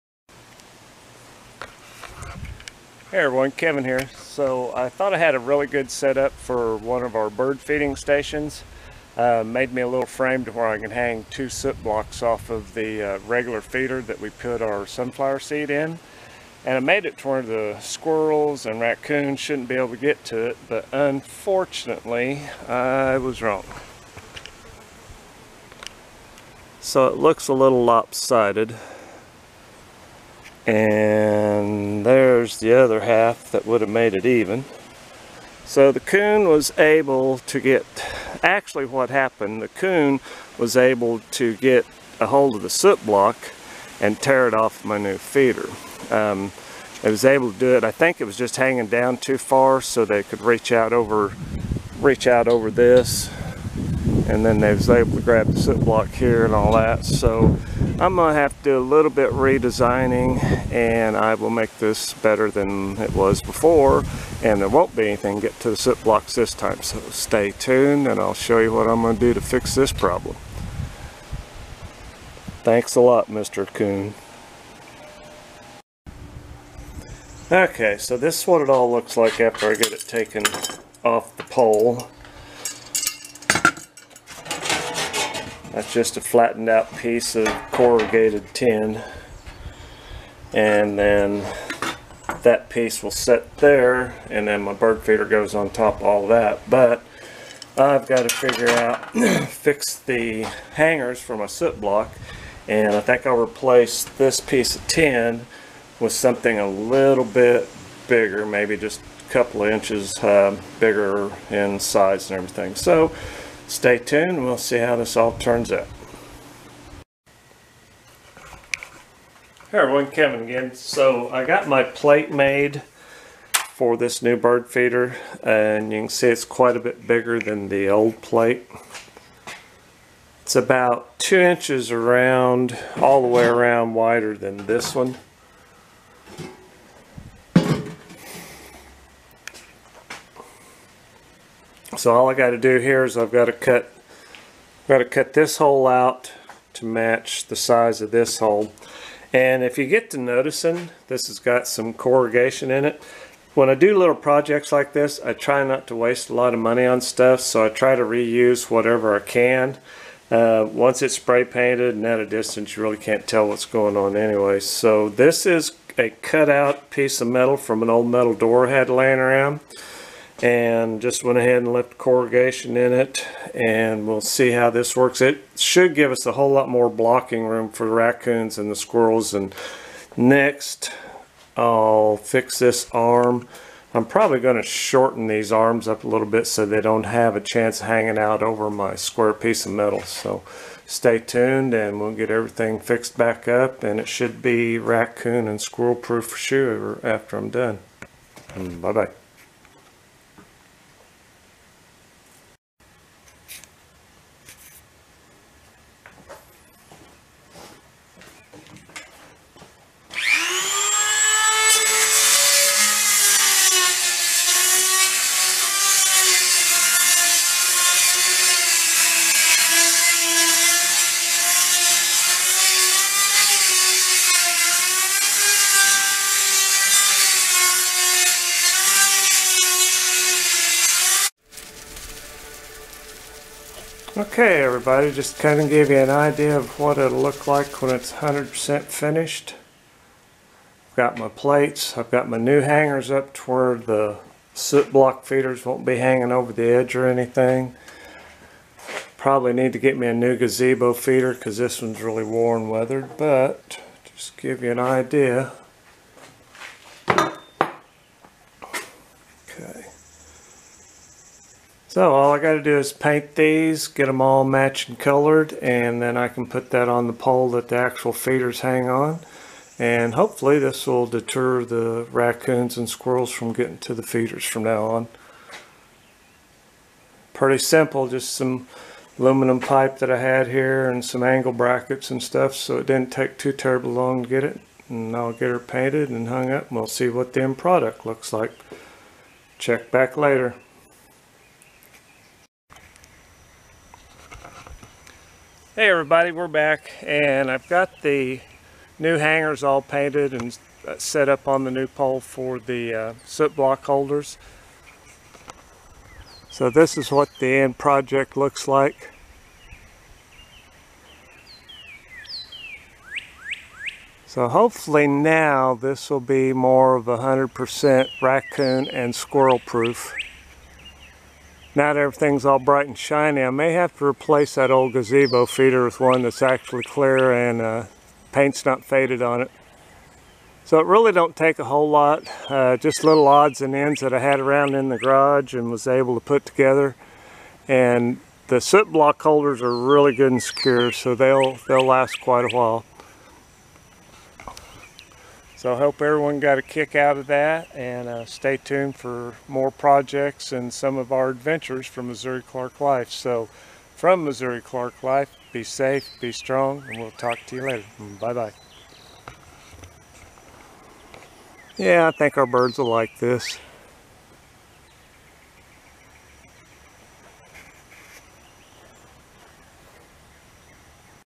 Making new feeder, Raccoons broke the first one.
After a Raccoon broke this first suet block holder I hope I made a better one. One I hope they can not reach this time.